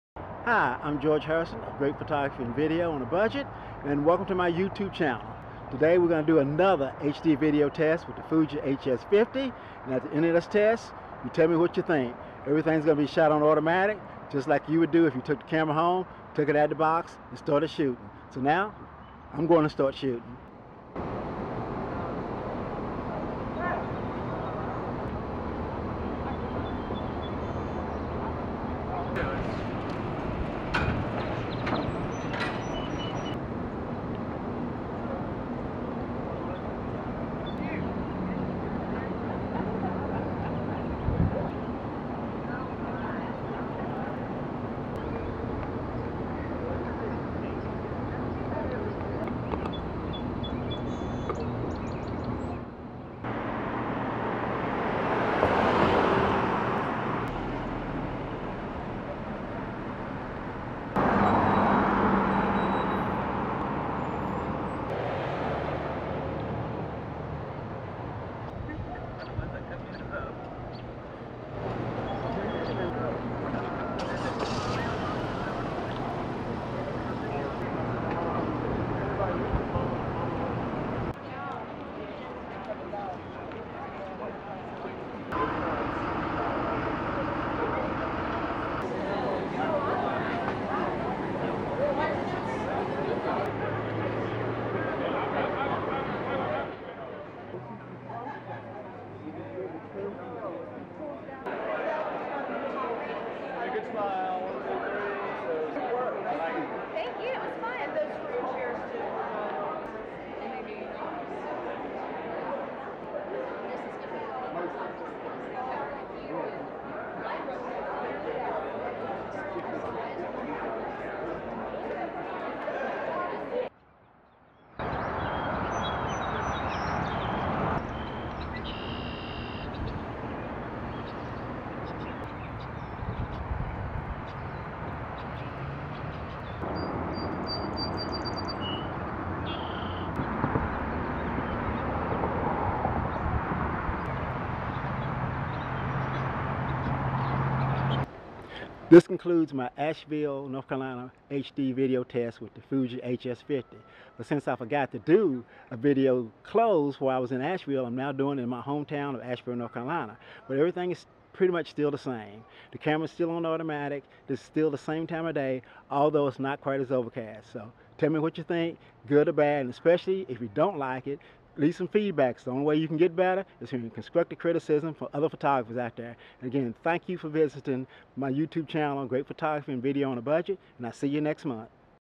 FUJI HS50 HD VIDEO MAGGIE VALLY.NC
Shot in the mountains of North Carolina in and around Maggie Valley and Lake Junaluska. All video was shot in ETX  mode straight out of the camera, tell me what you think.